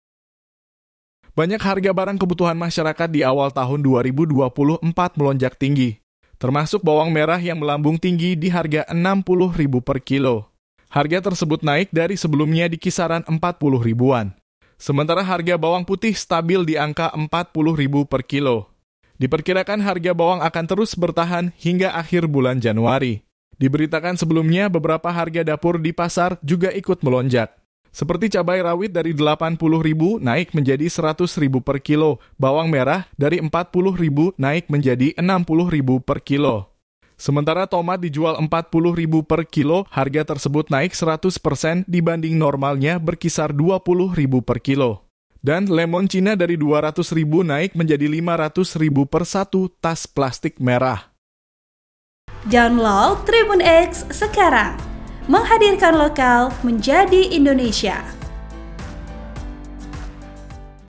Pekan Pertama 2024, Bawang Merah di Pasar Madika Ambon Naik Rp 60 Ribu
AL,  Wartawan TribunAmbon.com, Jenderal Louis
AMBON, TRIBUNAMBON.COM - Banyak harga barang kebutuhan masyarakat di awal tahun 2024 melonjak tinggi.
Termasuk Bawang Merah yang melambung tinggi di harga  60 ribu per kilo.
Harga tersebut naik dari sebelumnya di kisaran 40 ribuan.
Sementara harga Bawang Putih stabil di angka 40 ribu per kilo.